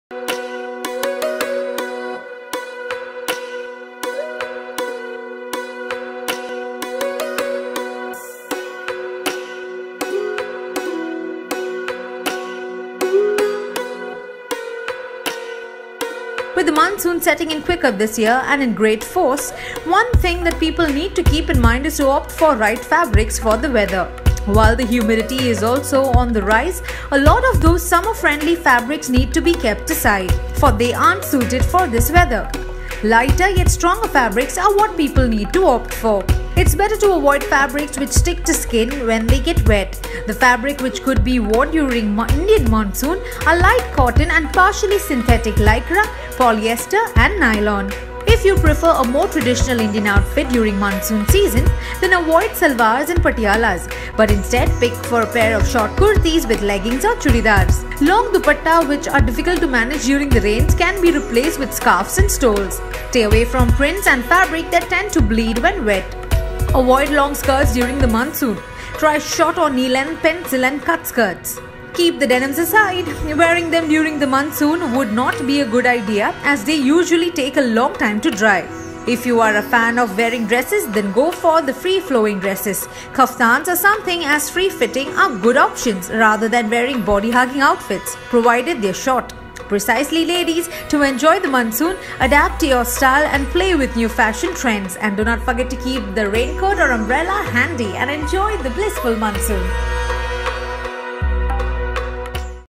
5 Evergreen Monsoon Trends | Monsoon Wardrobe Update | TSC inStyle
With the monsoon setting in quicker this year and in greater force, one thing that people need to keep in mind is to opt for the right fabrics for the weather. While the humidity is also on the rise, a lot of those summer-friendly fabrics need to be kept aside, for they aren't suited for this weather. Lighter, yet stronger, fabrics are what people need to opt for.